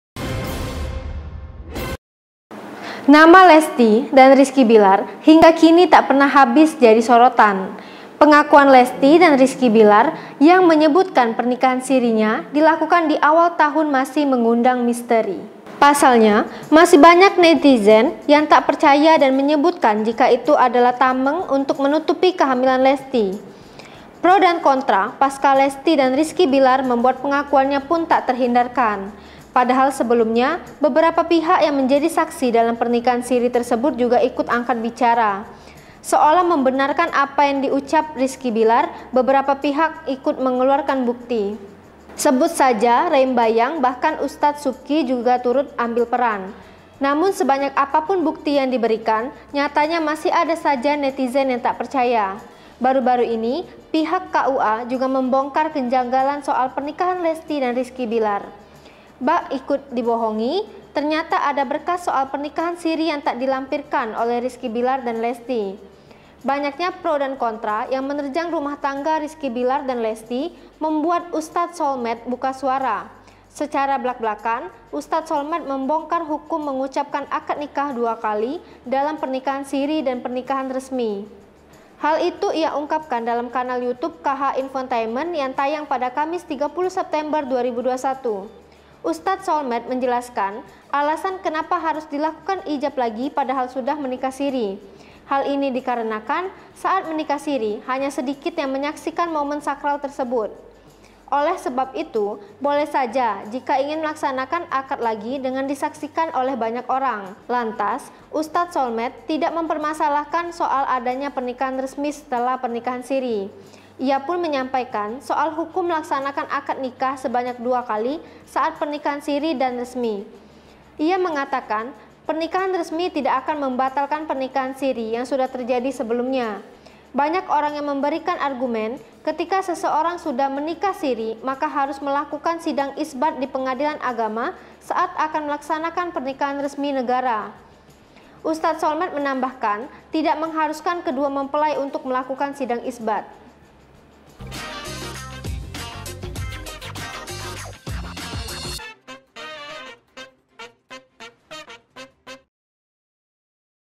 Ustaz Solmed Bongkar Hukum Akad 2 Kali yang Dilakukan Rizky Billar dan Lesty
Sebut saja Rey Mbayang bahkan Ustaz Subki juga turut ambil peran.

Namun sebanyak apapun bukti yang berikan, nyatanya masih ada saja netizen yang tak percaya.

Baru-baru ini pihak KUA juga membongkar kejanggalan soal pernikahan Lesty dan Rizky Billar.

Bak ikutan dibohongi, ternyata ada berkas soal pernikahan siri yang tak dilampirkan oleh Rizky Billar dan Lesty.

Banyaknya pro dan kontra yang menerjang rumah tangga Rizky Billar dan Lesty, membuat Ustaz Solmed buka suara.

Secara blak-blakan Ustaz Solmed membongkar hukum mengucapkan akad nikah dua kali, dalam pernikahan siri dan pernikahan resmi.

Hal itu ia ungkapkan dalam kanal YouTube KH Infotainment yang tayang pada Kamis (30/9/2021).

Ustaz Solmed menjelaskan alasan kenapa harus dilakukan ijab lagi padahal sudah menikah siri.

Hal ini dikarenakan, saat menikah siri hanya sedikit yang menyaksikan momen sakral tersebut.

Oleh sebab itu, boleh saja jika ingin melaksanakan akad lagi dengan disaksikan oleh banyak orang.